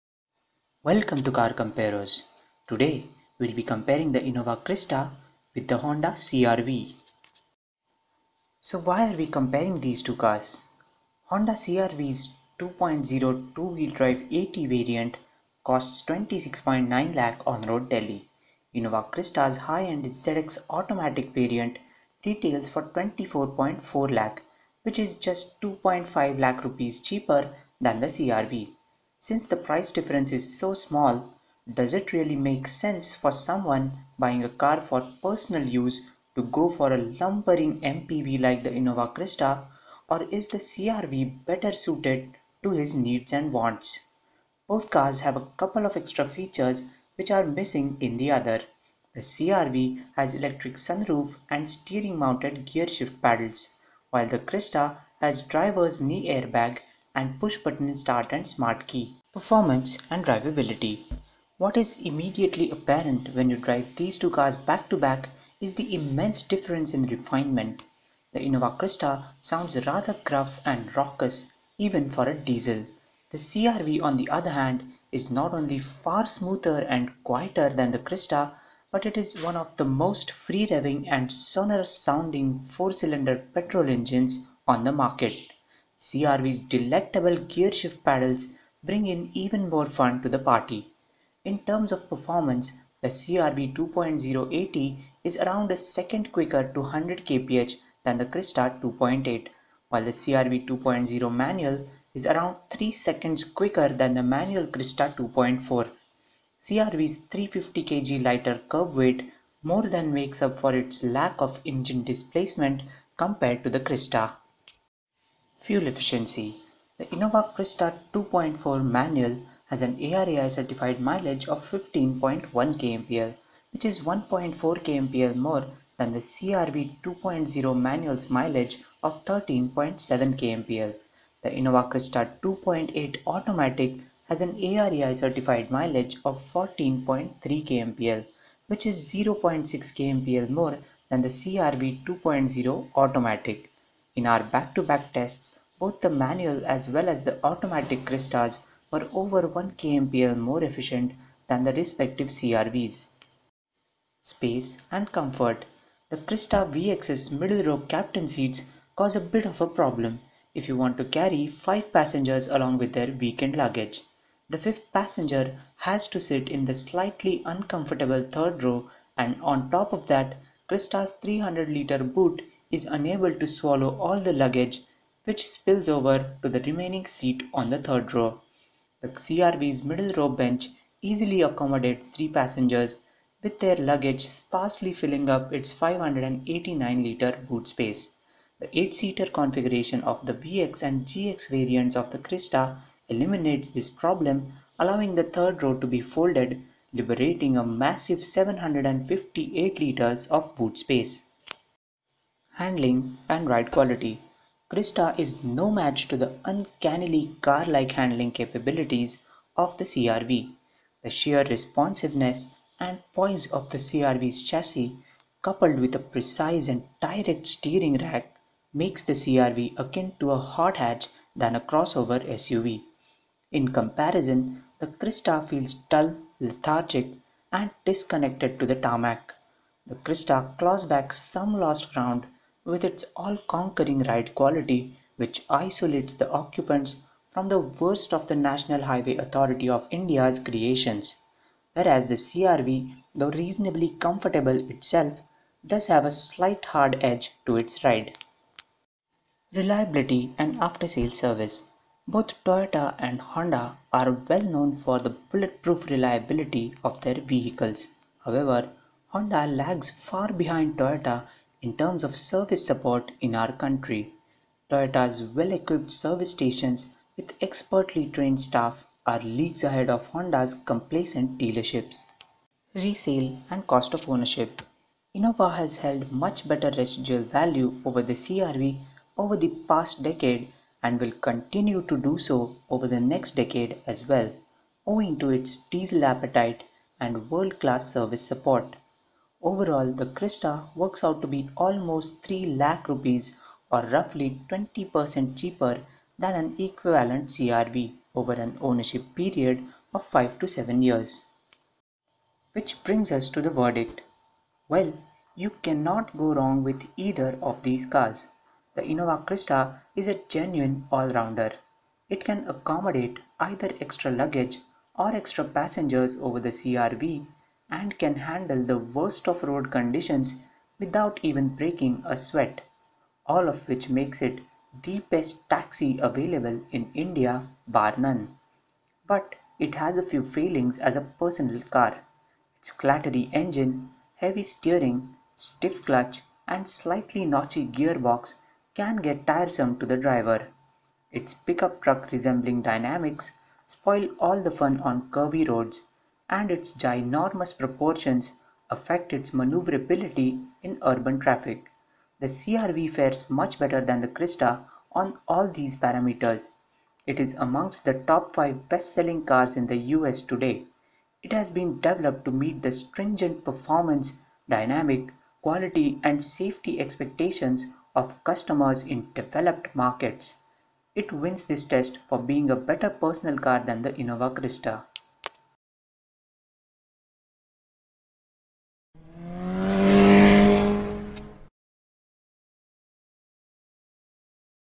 Innova Crysta vs Honda CRV Comparison Review
Honda CR-V vs Toyota Innova Crysta Comparison, with detailed price, mileage, features, performance, handling, ride quality, space, comfort, cost of ownership and after sales service comparison.
To check out the facts and figures, head over to CarComparos.com by following the link given below